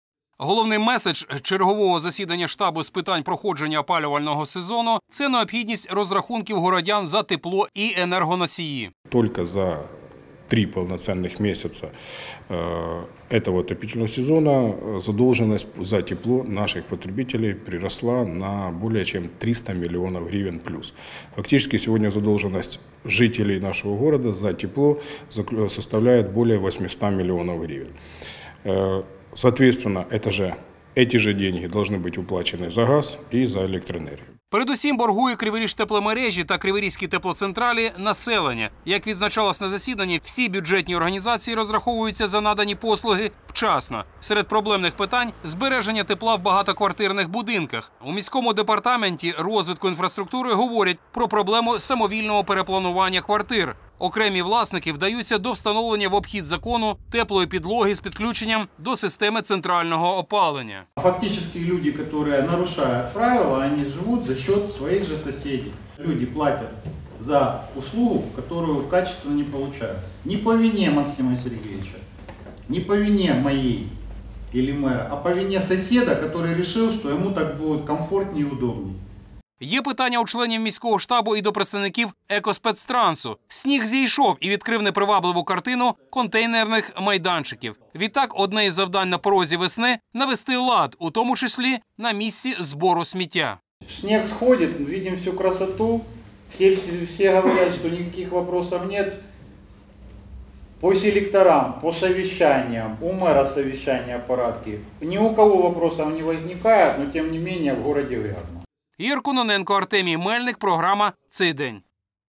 Штаб по зимі
Заборгованість споживачів тепла перед обома теплогенеруючими підприємствами, робота управителів, інші питання проходження опалювального сезону. Про це йшла мова сьогодні на засіданні відповідного міського штабу. Докладніше - у сюжеті.